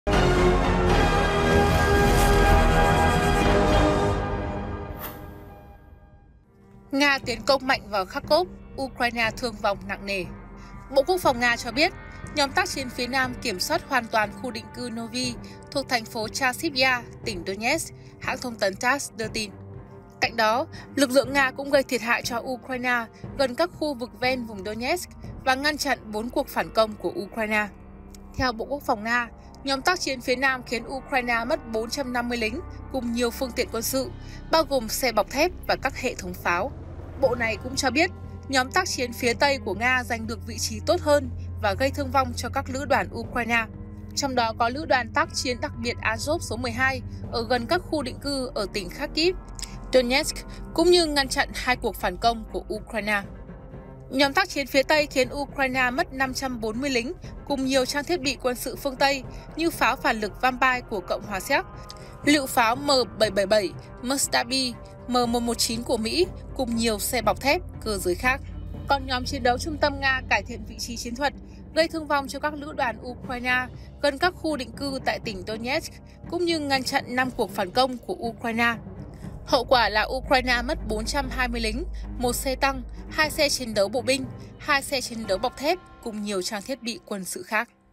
Nga tiến công mạnh vào Kharkov, Ukraine thương vong nặng nề | Báo Công Thương
Báo Công Thương | Bộ Quốc phòng Nga cho biết Nhóm tác chiến phía Nam kiểm soát hoàn toàn khu định cư Novy thuộc TP Chasiv Yar (tỉnh Donetsk), hãng thông tấn TASS đưa tin.